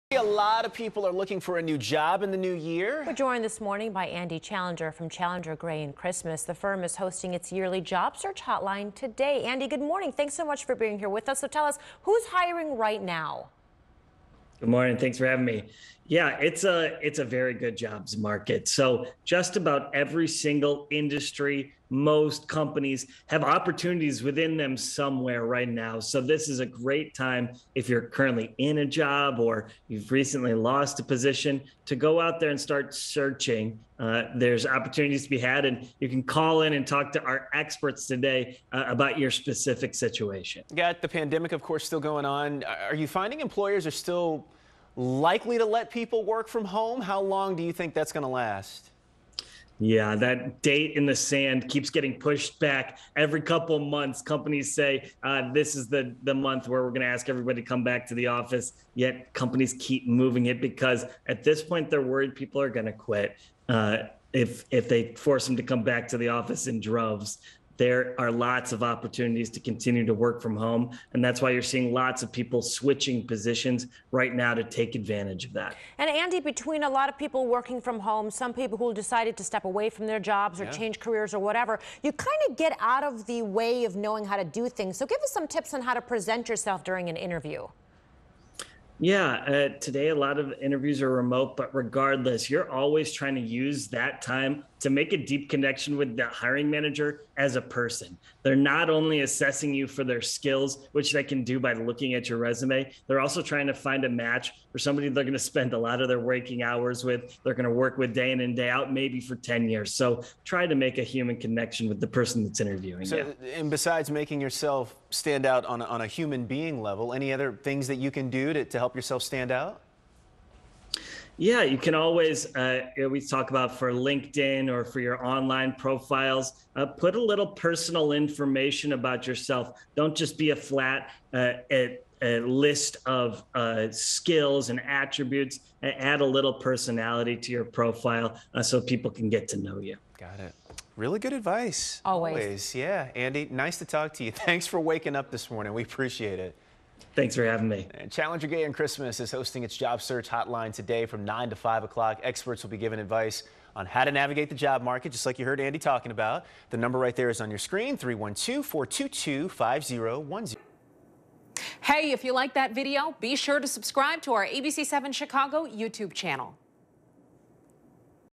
Job search tips from Challenger, Gray and Christmas
A lot of people are looking for a new job in the New Year.